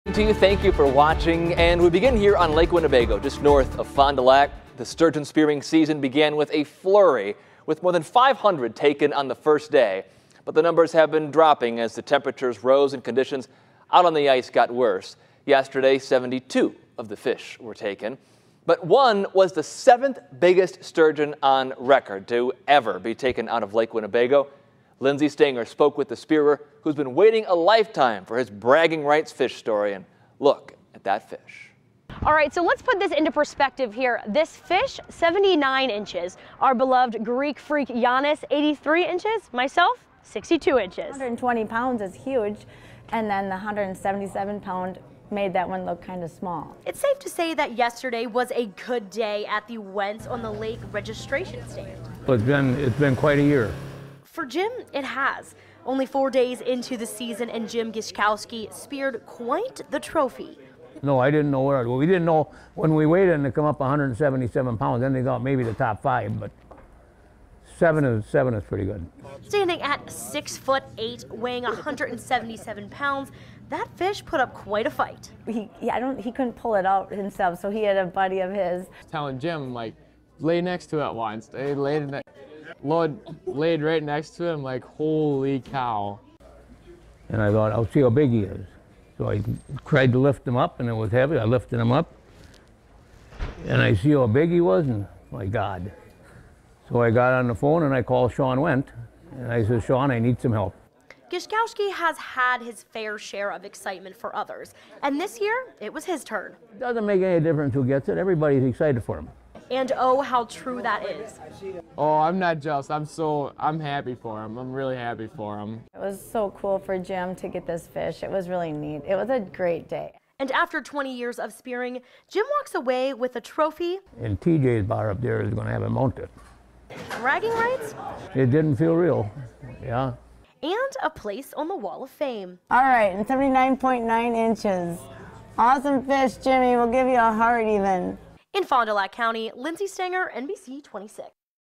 7th largest sturgeon speared on Lake Winnebago
In the small town of Van Dyne, a local bar, and grill received the surprise of a lifetime when Jim Gishkowsky, a local and regular, pulled out a 177 lbs sturgeon this season.